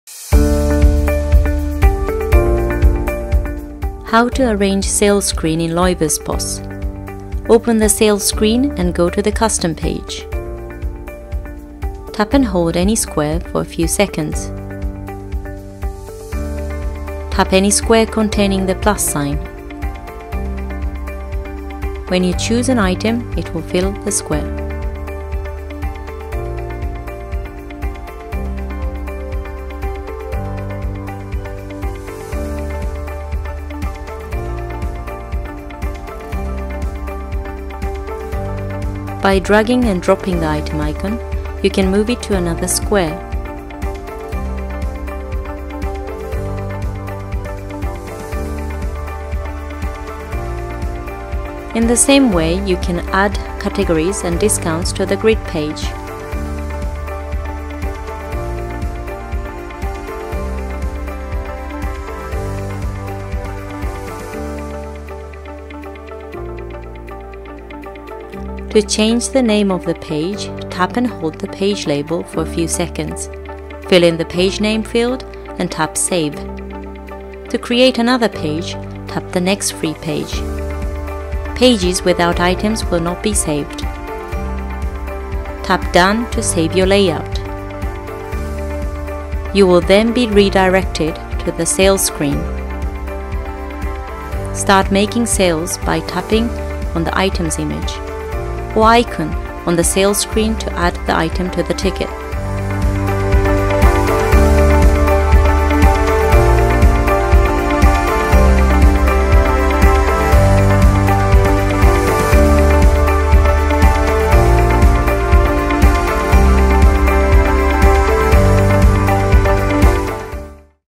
How to Arrange Sale Screen in Loyverse POS System
You can arrange your items on the sales screen of Loyverse POS in a suitable way by creating new custom pages and organizing the items there